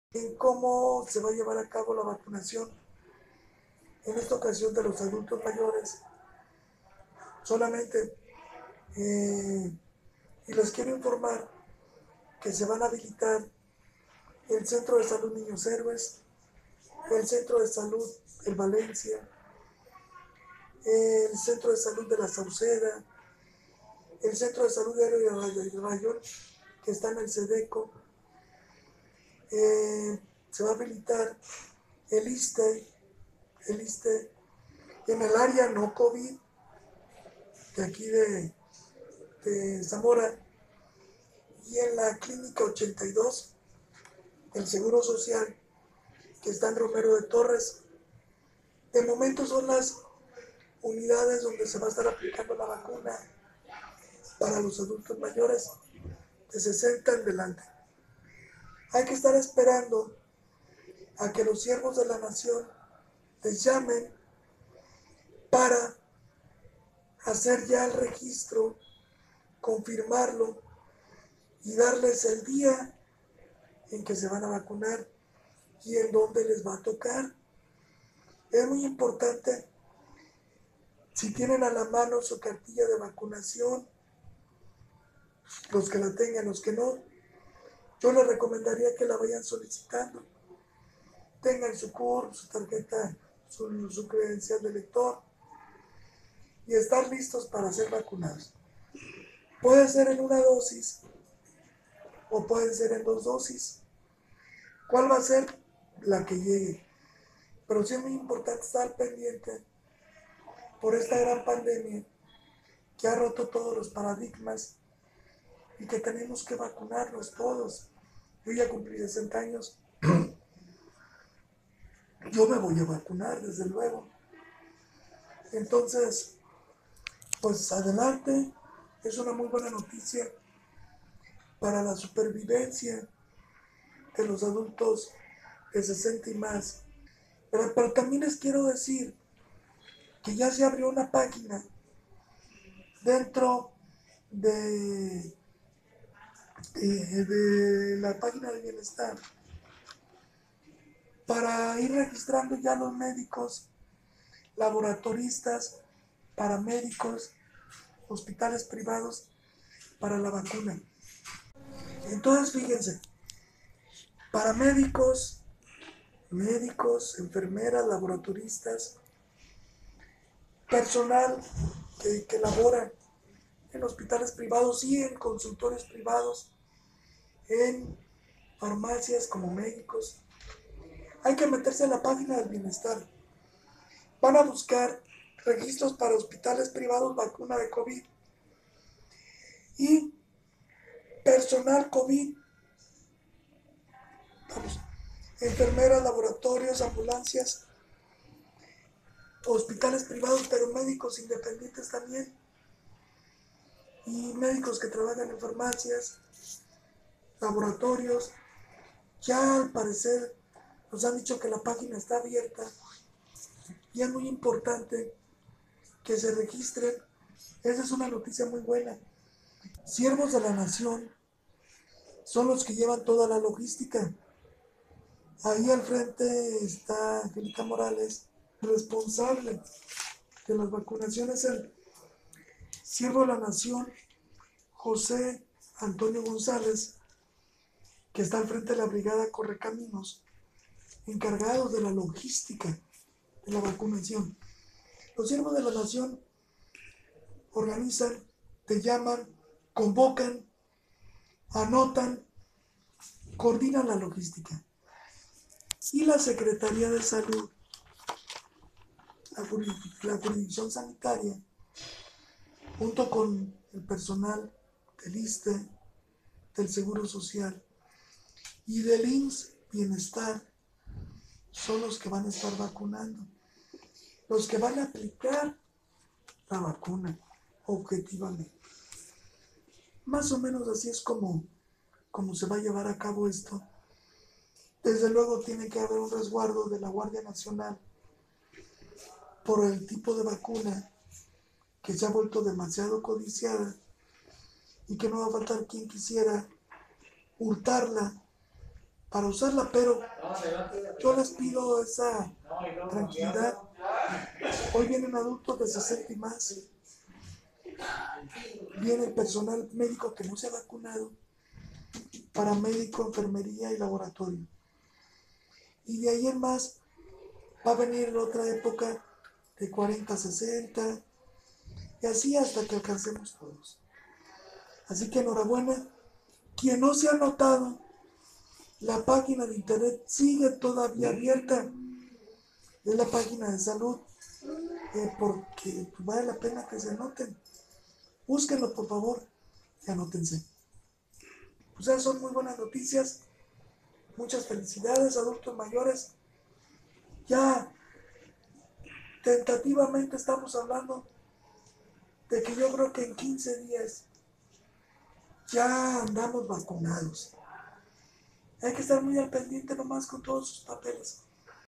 Pronto llegan vacunas para adultos y personal de salud en Zamora
Lugares donde se aplicarán.
Para personal de salud  publicos, privados y adultos mayores.